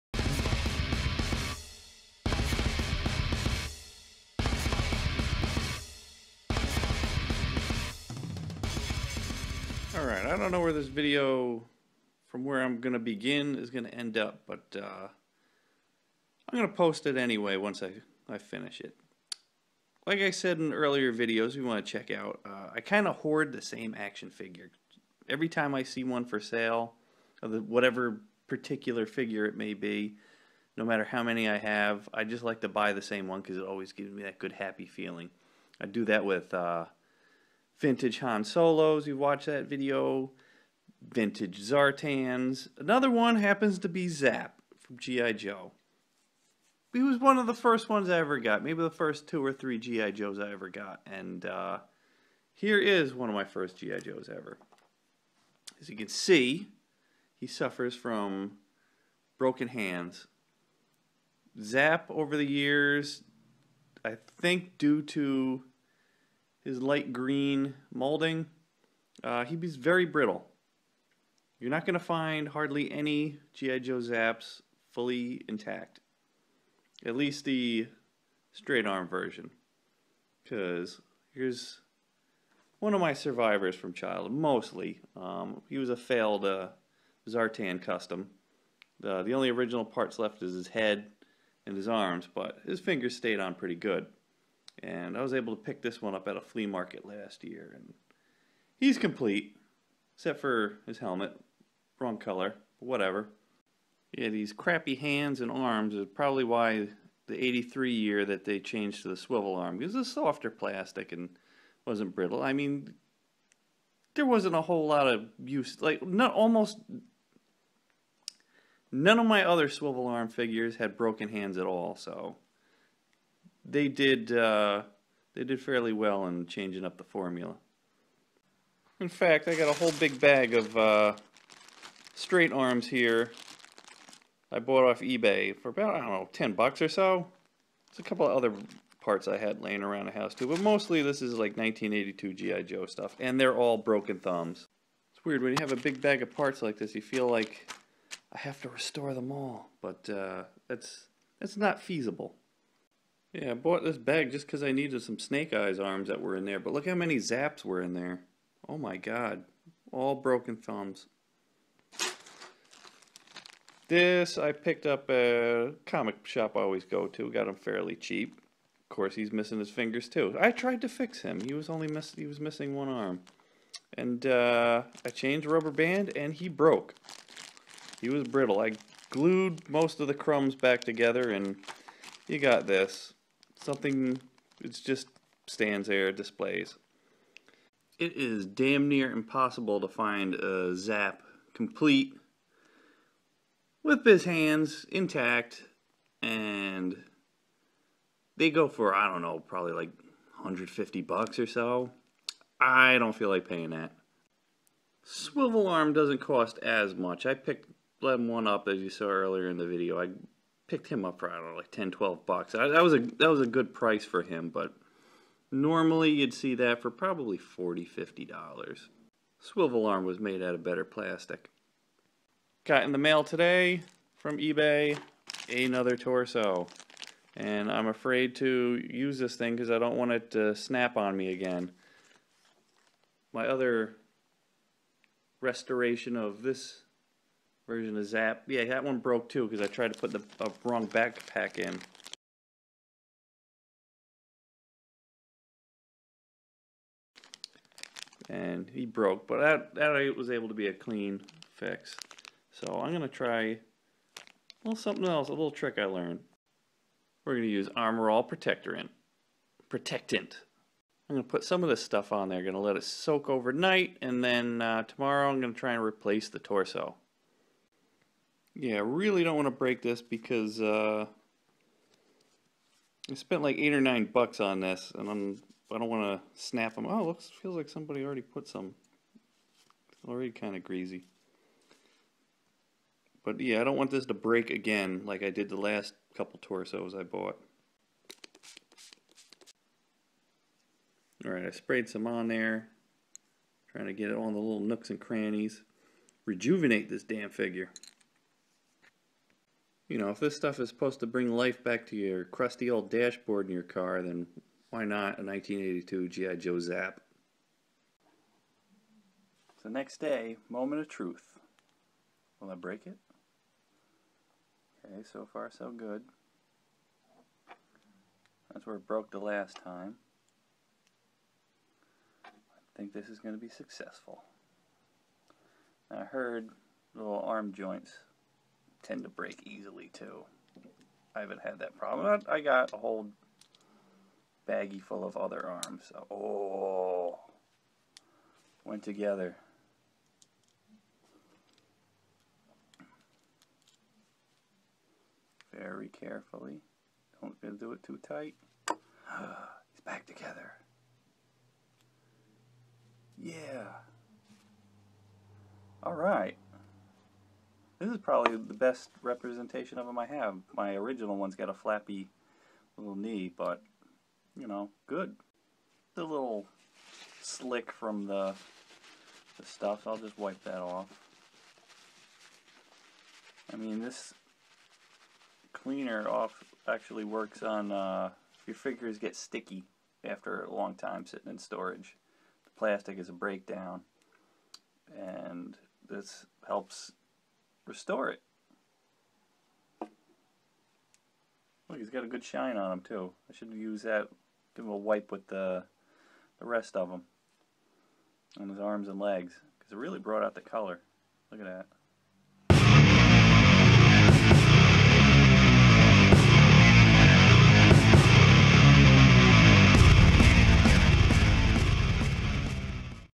1982 GI JOE ZAP & using Armor-All on brittle and broken figures - MIKE PLAYS WITH VINTAGE TOYS #86
MIKE PLAYS WITH VINTAGE TOYS #86

The first wave of GI Joe figures didn't age well. Hell, most of them broke way back in 1982 and that's why they got a redesign in 1983 because all their thumbs and fingers broke any time you tried to put a weapon in their little hands. The most notorious was Zap! He had two problems against him. His shade of green plastic somehow made him more susceptible to breakage and he had a giant bazooka handle to cram in between his fingers.

With time the rest of the figure is pretty brittle two as I broke 3 of them.

Here I try to restore a Zap after a few failed attempts using replacement parts and trying out Armor-All to protect and rejuvenate 35 year old plastic!

The idea I got using Armor-all on action figures comes from this video...